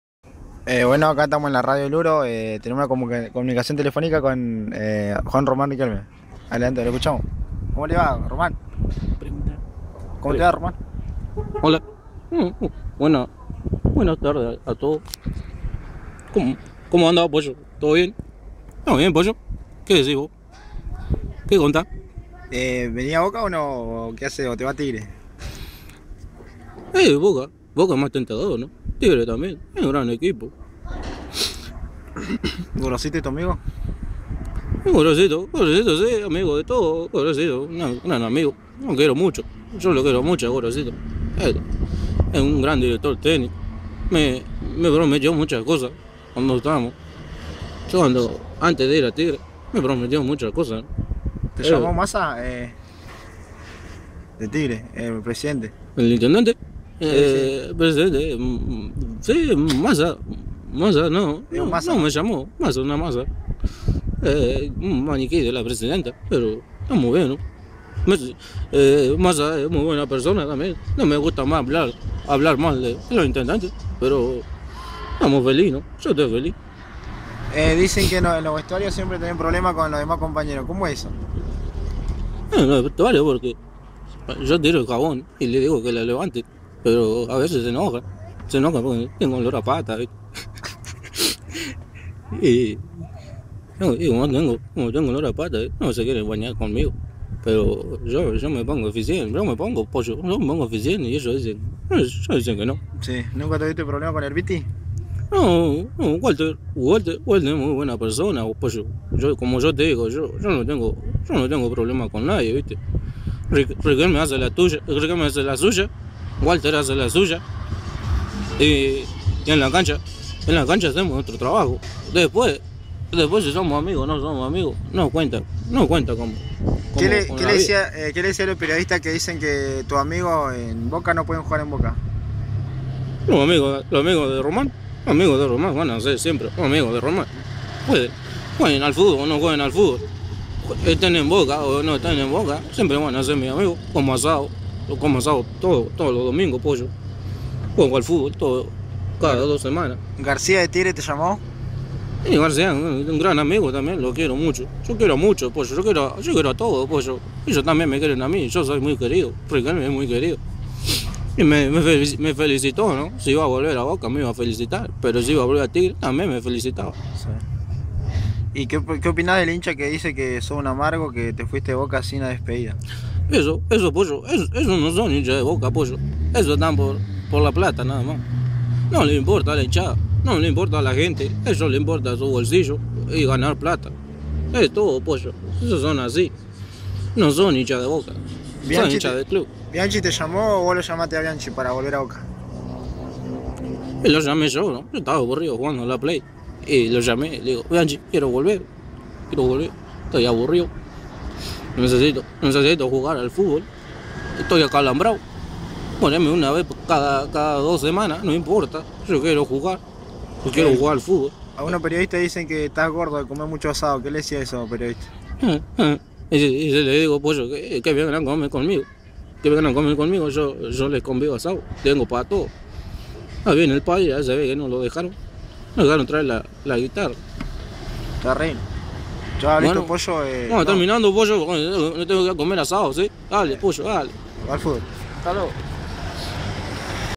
la vuelta de roman a boca..
nos lo crusamo x la calle y el respondio..!